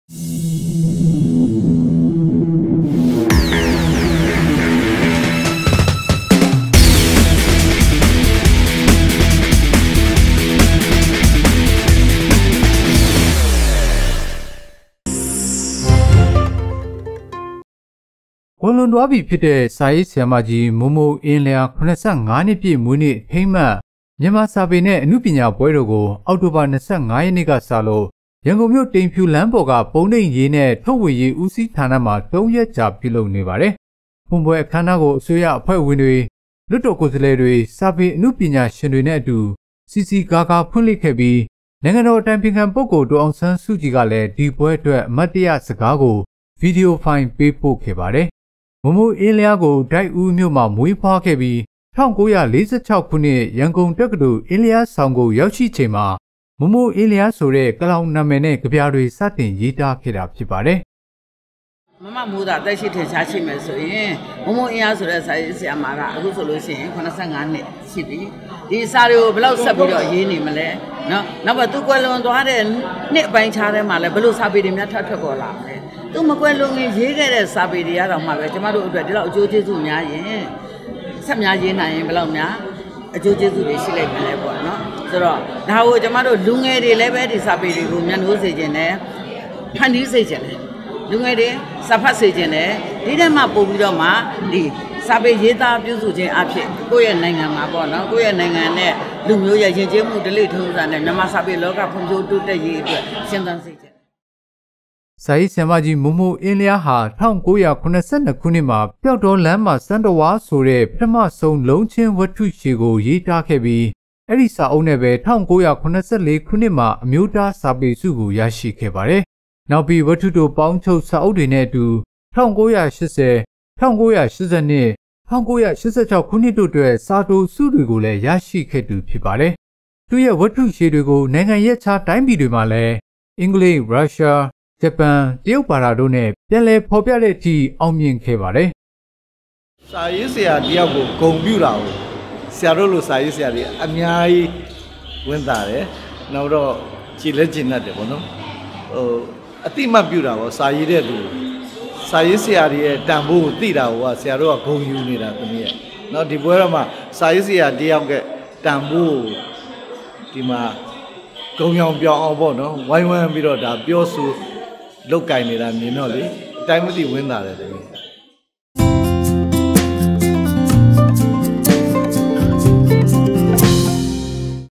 မိုးမိုး(အင်းလျား) ၇၅နှစ်ပြည့် မြန်မာစာပေနှင့်အနုပညာပွဲတော်
မိုးမိုး(အင်းလျား) ၇၅နှစ်ပြည့် မြန်မာစာပေနှင့်အနုပညာပွဲတော် (News Video)

“မမမိုးသာ သက်ရှိထင်ရှားရှိနေမယ်ဆိုရင် အခုဆို အသက် ၇၅နှစ်ရှိပြီ။ ဒီစာတွေကို ဘယ်လောက်ထိဆက်ပြီးတော့ ရေးနေမလဲ။ သူကွယ်လွန်သွားတဲ့ နှစ်အပိုင်းအခြားမှာလည်း ဘယ်လိုစာပေများ ထပ်ထွက်ပေါ်လာမလဲ..”

[Zawgyi]
မိုးမိုး(အင္းလ်ား) ၇၅ႏွစ္ျပည့္ ျမန္မာစာေပႏွင့္အႏုပညာပြဲေတာ္ (News Video)

“မမမိုးသာ သက္ရွိထင္ရွားရွိေနမယ္ဆိုရင္ အခုဆို အသက္ ၇၅ႏွစ္ရွိၿပီ။ ဒီစာေတြကို ဘယ္ေလာက္ထိဆက္ၿပီးေတာ့ ေရးေနမလဲ။ သူကြယ္လြန္သြားတဲ့ ႏွစ္အပိုင္းအျခားမွာလည္း ဘယ္လိုစာေပမ်ား ထပ္ထြက္ေပၚလာမလဲ..”